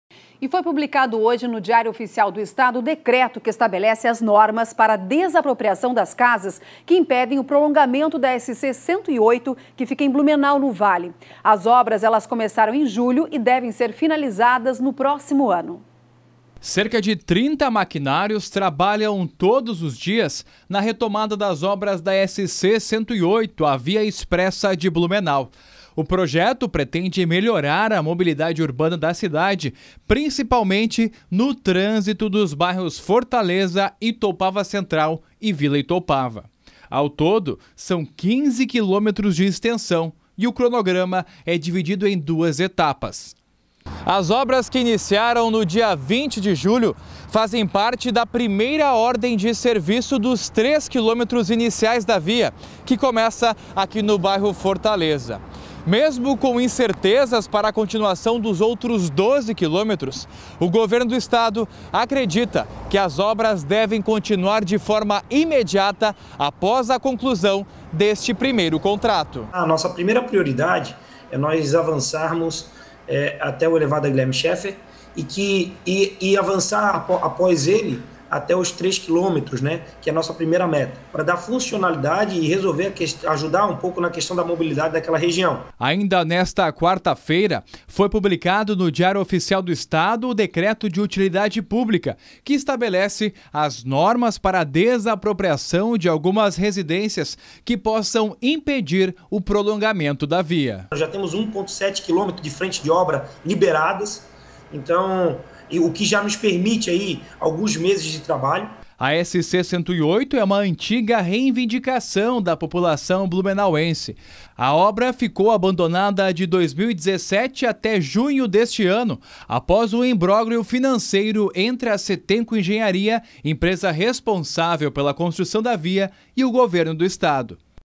Governo publica decreto com regras para desapropriação de casas que impedem obras em Blumenau
Foi publicado nesta quarta-feira (1), no Diário Oficial do Estado, o decreto que estabelecer as normas para a desapropriação das casas que impedem obras na SC-108.

Confira.